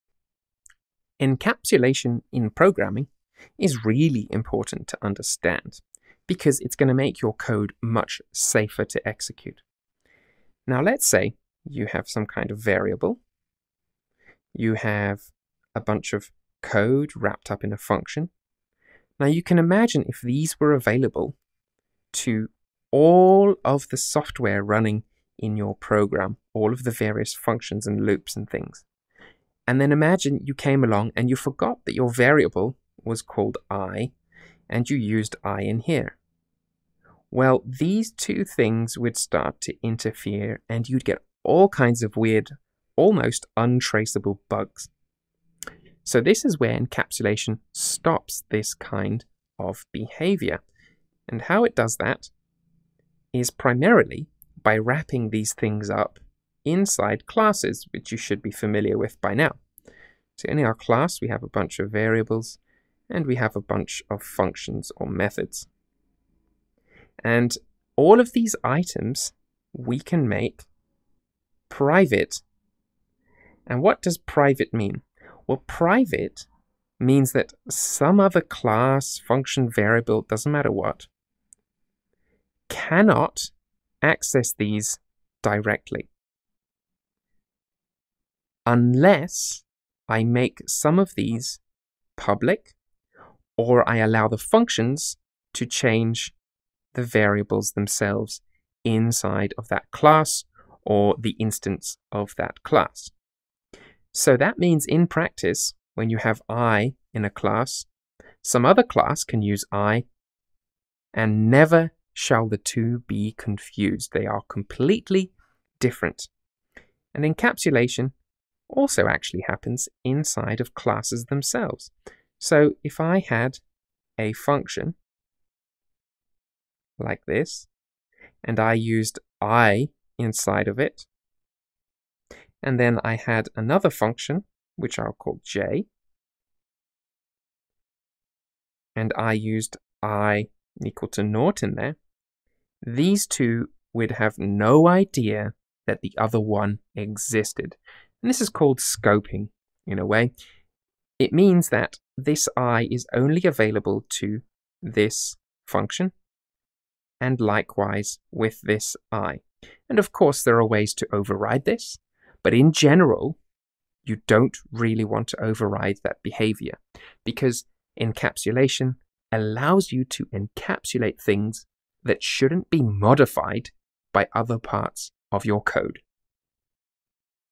What is encapsulation in programming?
Get "Ultimate bGuide to Software Freelancing" - a FREE roadmap for a very, very lucrative career.
Encapsulation is kind of like stuffing everything in one draw so you know where it is!